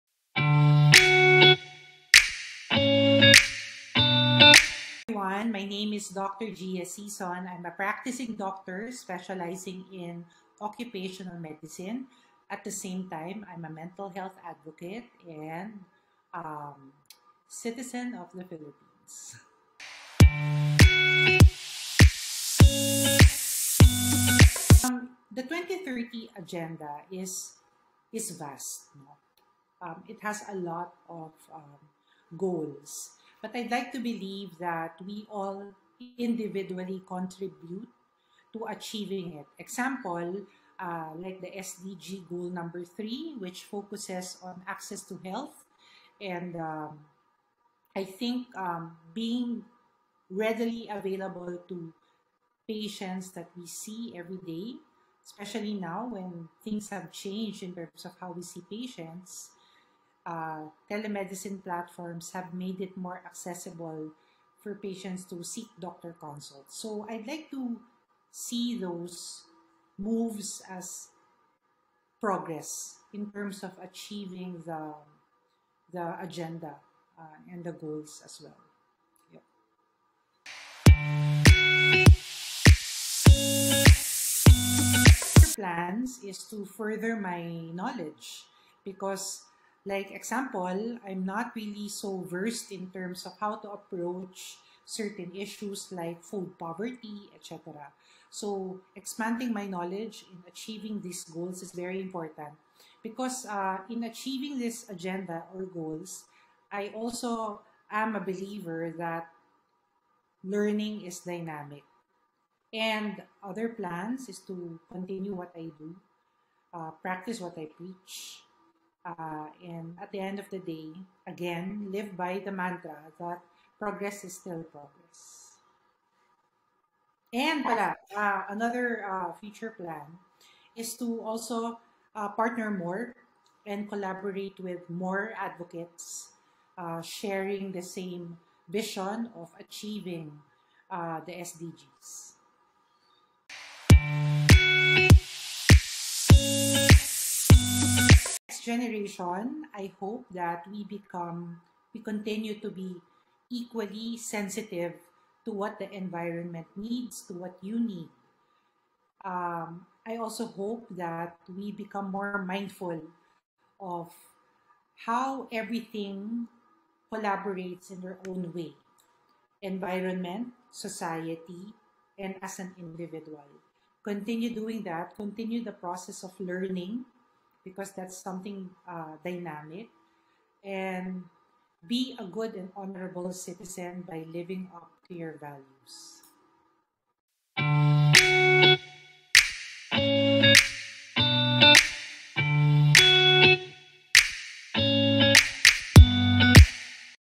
LOCAL STEWARD OF THE SDG: Dr. Gia Sison
Dr. Gia is a practicing medical doctor specializing in Occupational Medicine and is a graduate of the University of Sto. Tomas. She co-founded HealthXPh which promotes the responsible use of social media in healthcare amongst others, and launched a project called Padayon PH at padayonph.com which aims to serve as a mental health resource hub hoping that more people gain access to.